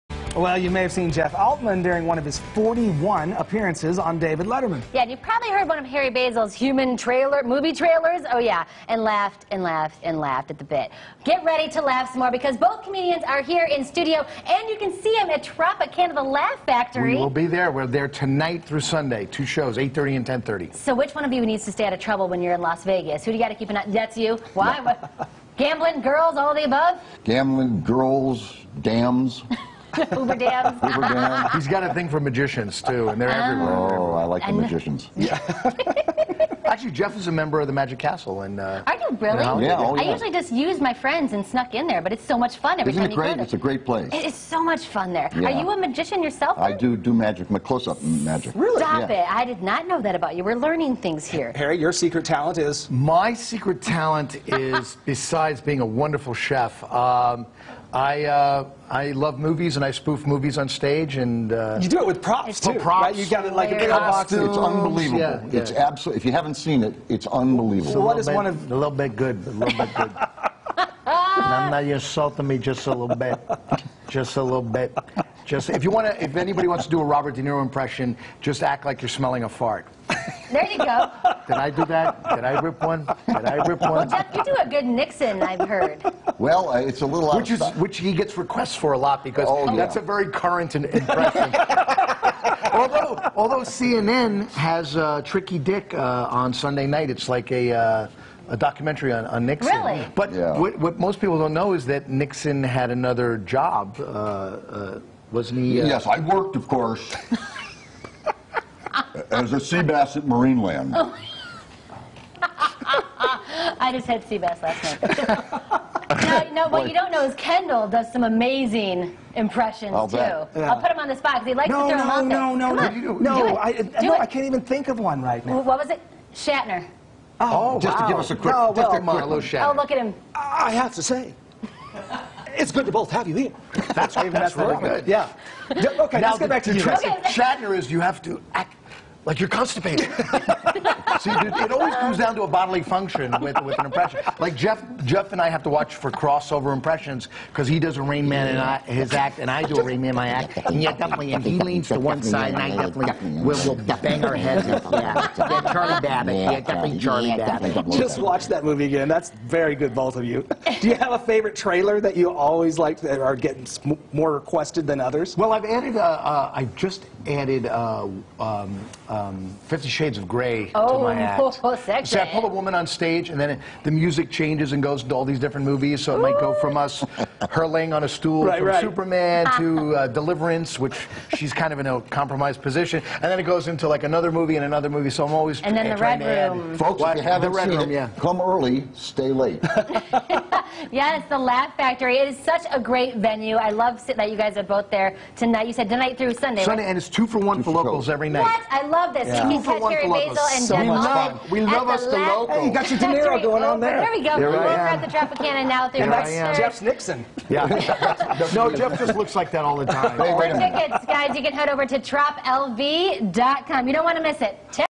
Comedians Harry Basil & Jeff Altman are live in-studio
Catch the comedians at The Laugh Factory through Sunday